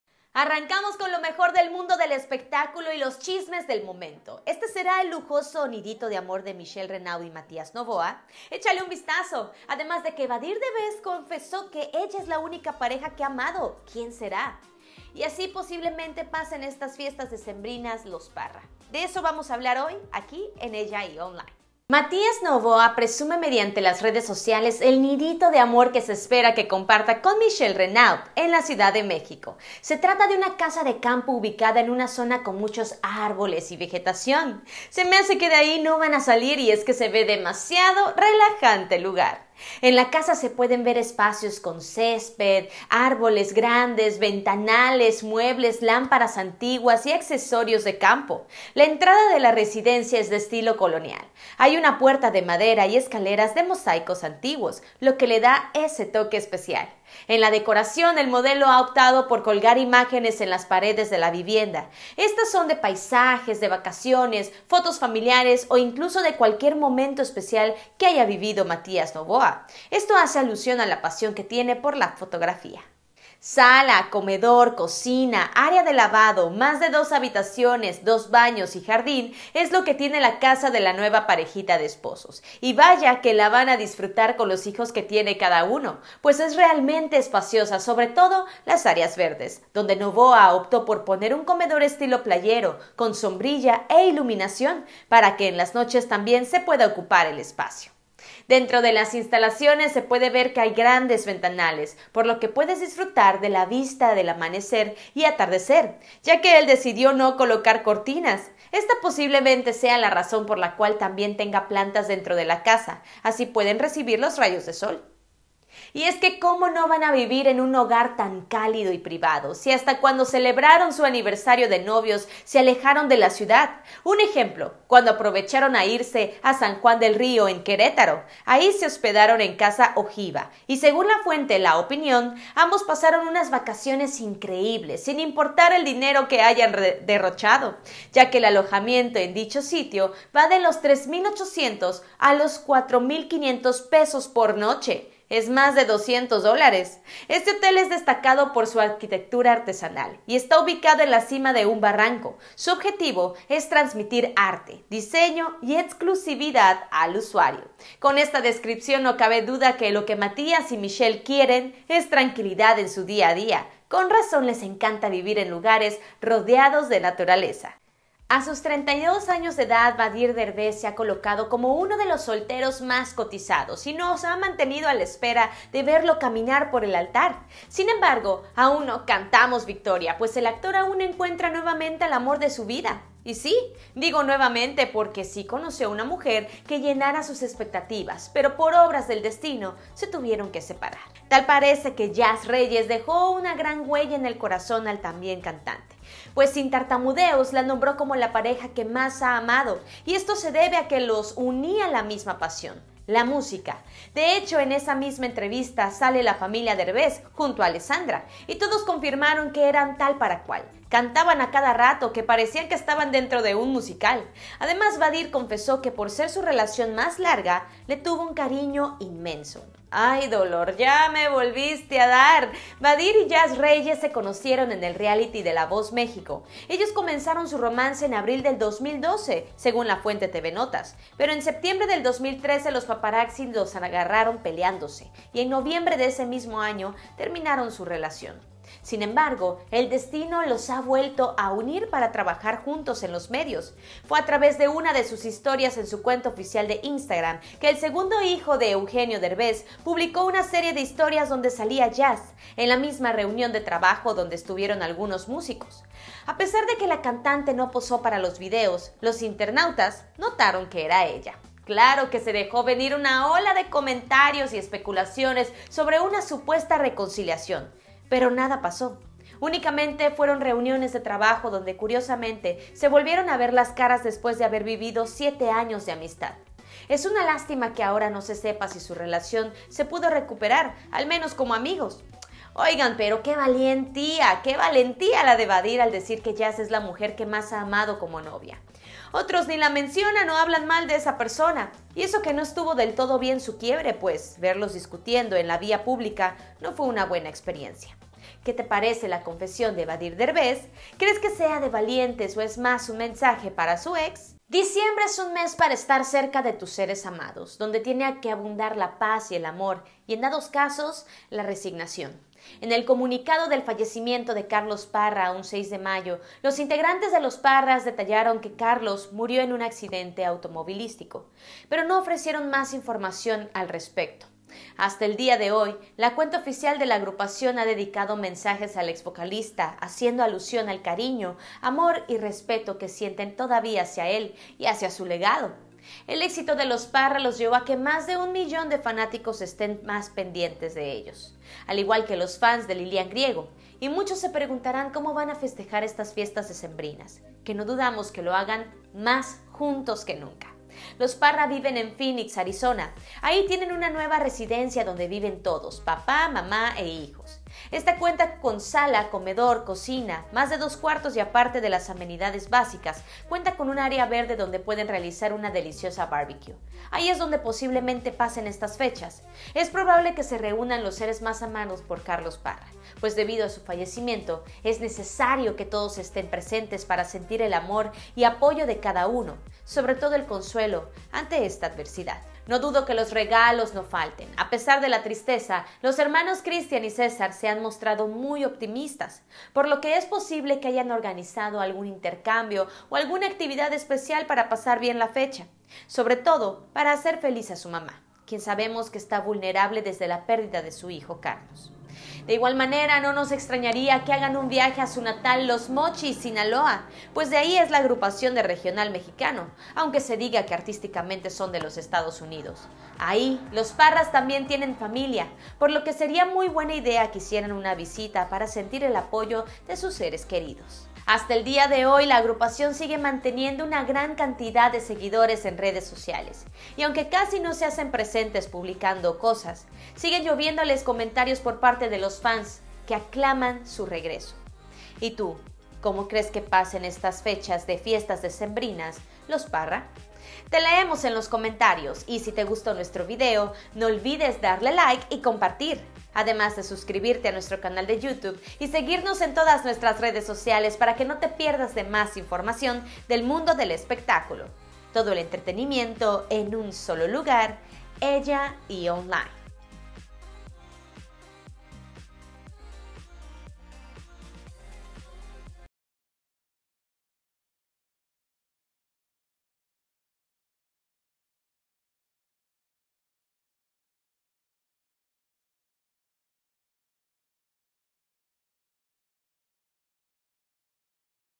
📍¡ÚLTIMAS NOTICIAS! Ella es la única pareja de VADHIR que amado/ Así pasarán los Parra estas fiesta
🚨ÚLTIMAS NOTICIAS🚨
-        El lujoso nidito de amor de Michelle Renaud y Matías Novoa
-        Ella es la única pareja de Vadhir Derbez que amado
-        Así pasarán los Parra estas fiesta navideñas sin Carlos Parra